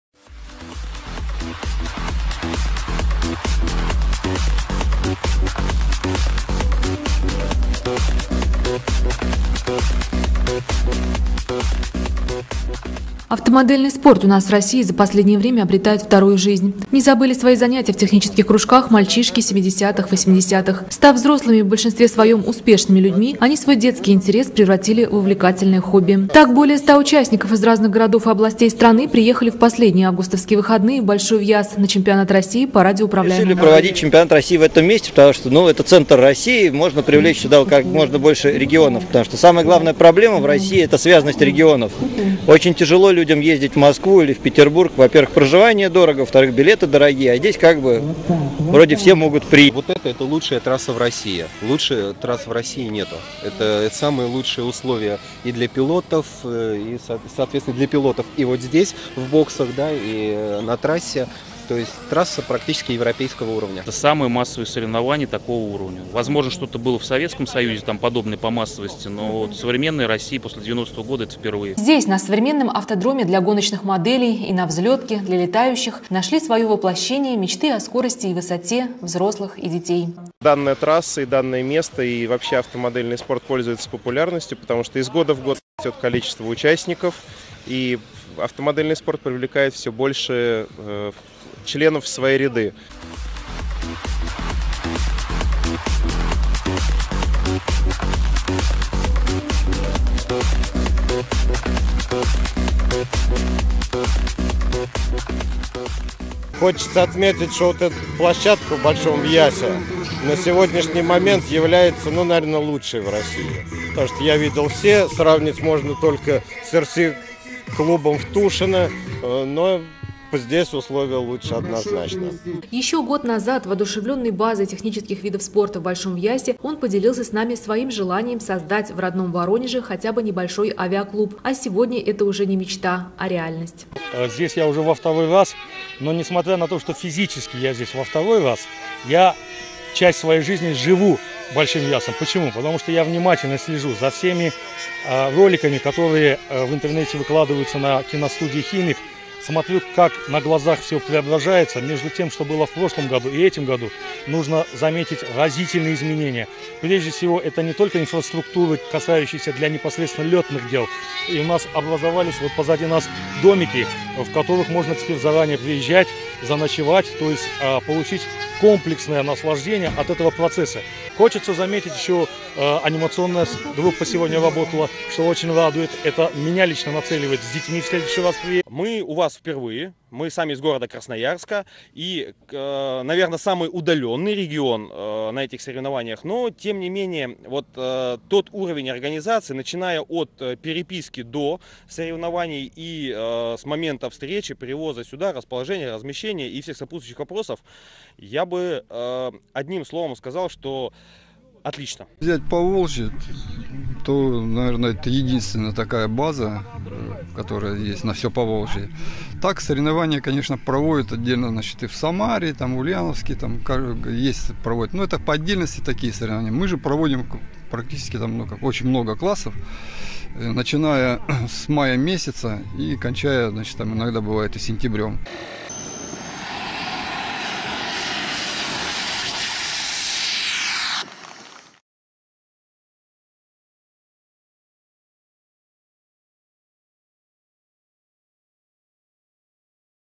Авто и авиамодельный спорт, Республика Мордовия
Данное видео представлено на IV-й туристский видеофестиваль "ДИВО РОССИИ". СТК "Радуга" (ИП Ивашкина М.А.). Номинация - Объекты приключений и событийного туризма